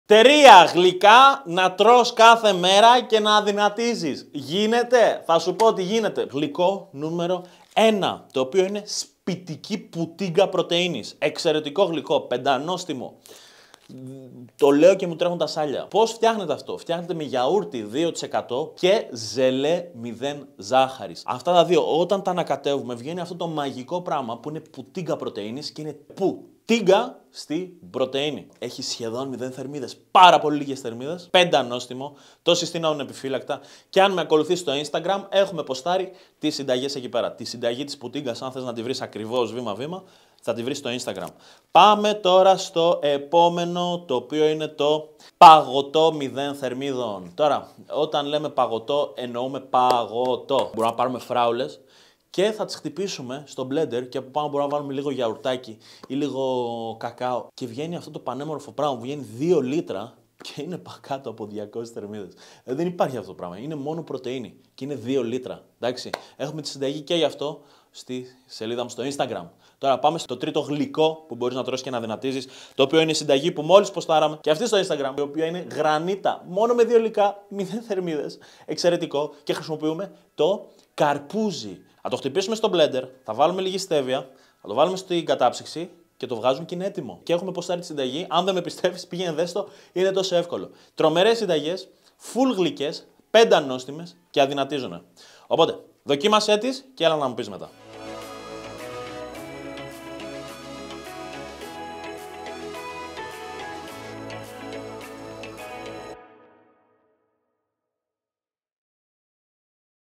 Ποια Γλυκά Αδυνατίζουν;;
Το καλοκαίρι πλησιάζει και είναι η τέλεια ευκαιρία να δοκιμάσεις γλυκά που σε αδυνατίζουν για να έχεις επίπεδη κοιλιά. Ανακάλυψε τα γλυκά που σε αδυνατίζουν για να απολαύσεις το καλοκαίρι χωρίς ενοχές και με επίπεδη κοιλιά. Κάνε το καλοκαίρι πιο γλυκό και ελαφρύ με αυτά τα υγιεινά γλυκά που σε αδυνατίζουν.

Πως Θα Χάσεις 10 Κιλά Λίπους Σε 3 Μήνες Μέσω Της Διαλειμματικής Νηστείας

Στείλε μου απορίες στο ινστα